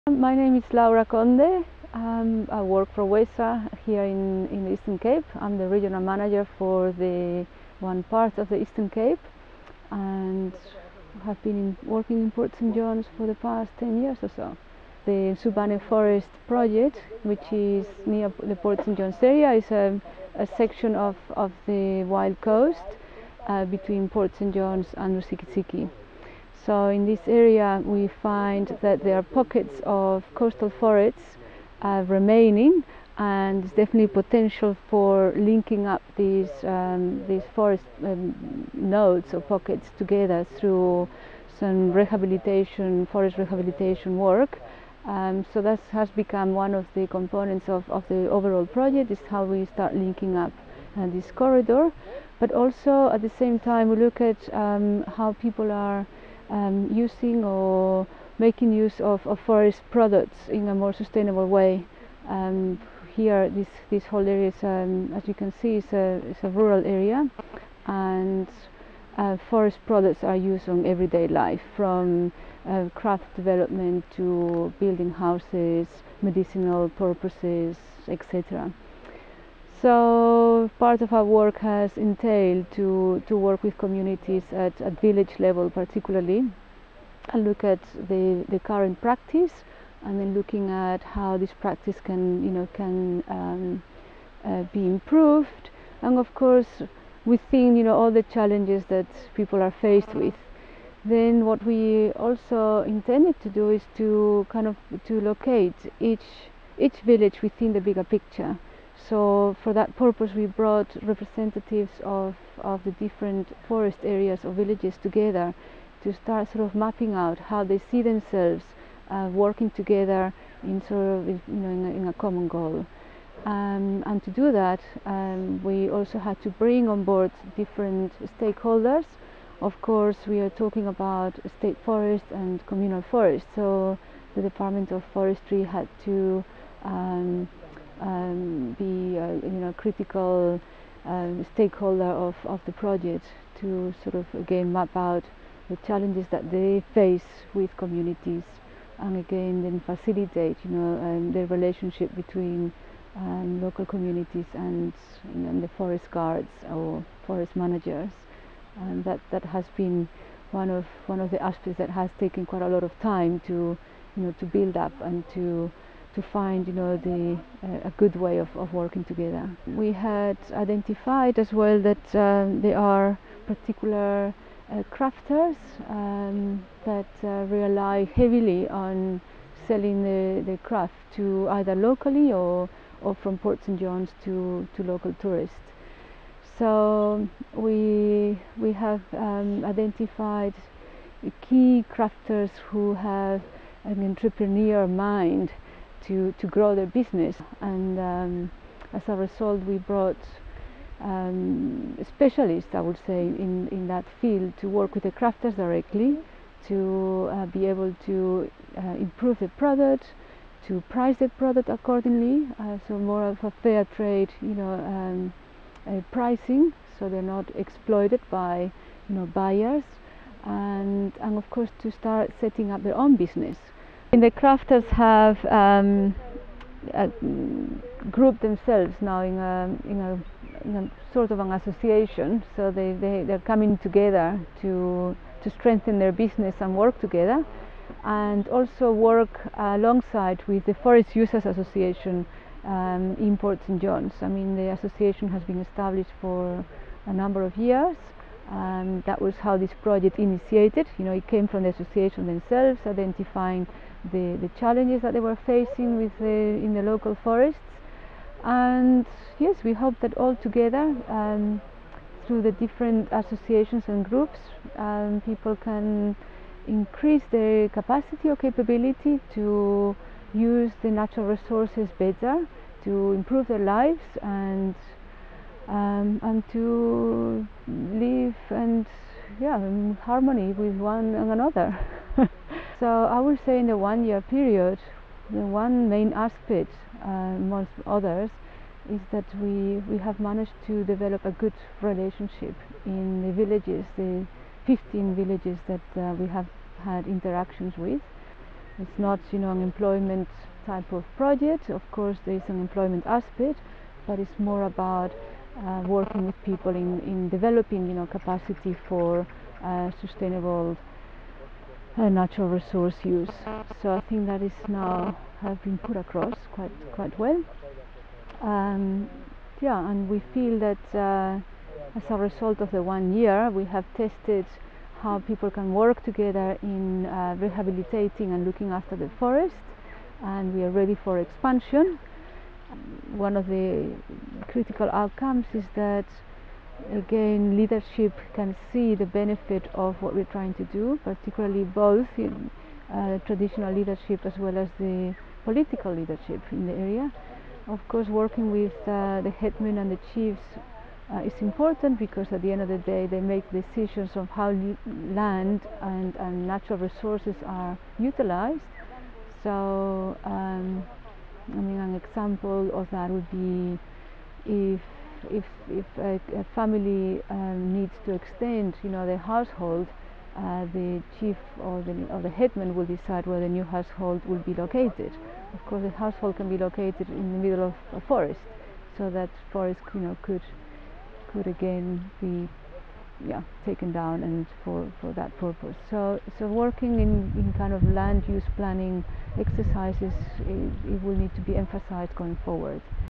How WESSA works with communities
Laura Conde of WESSA explains importance of the Ntsubane forests and their link to the livelihoods of nearby communities.